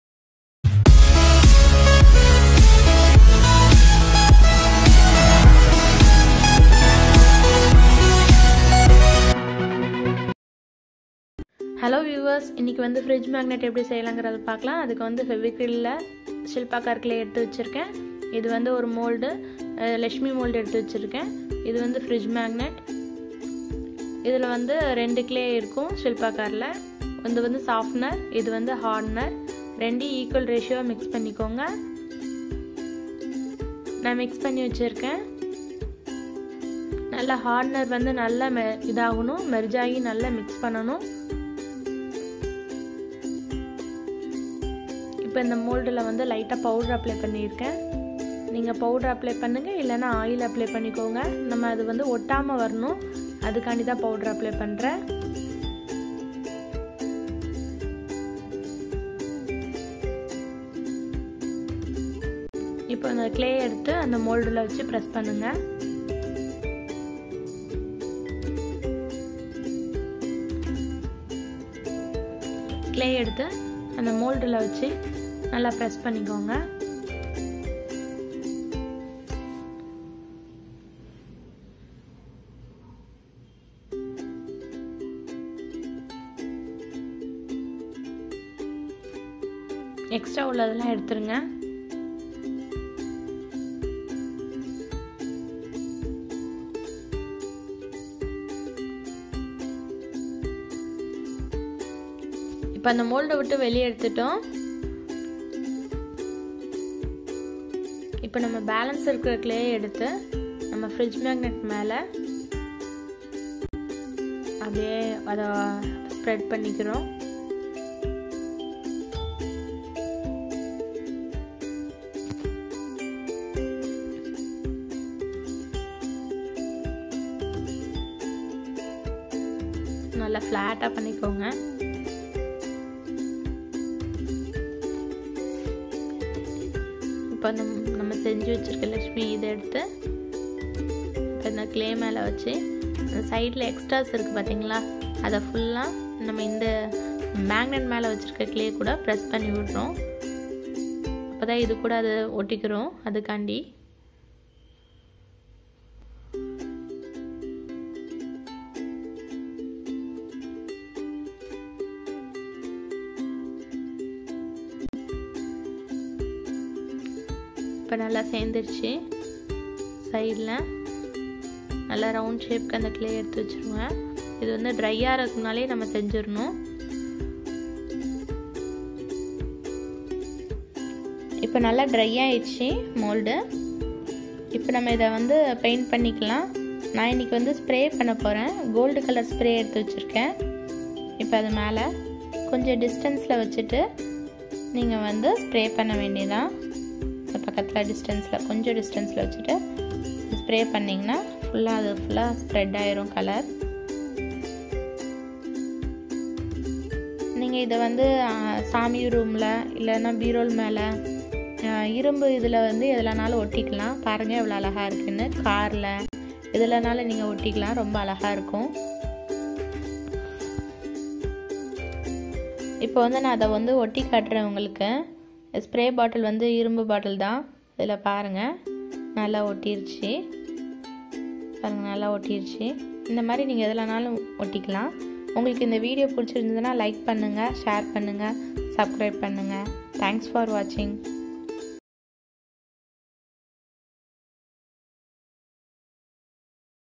FRIDGE MAGNET CRAFT | CLAY CRAFT | FEVICRYL SHILPKAR CRAFT
Required Material
1.Fridge Magnet
2.Fevicryl Shipkar Clay
3.Silicon Mould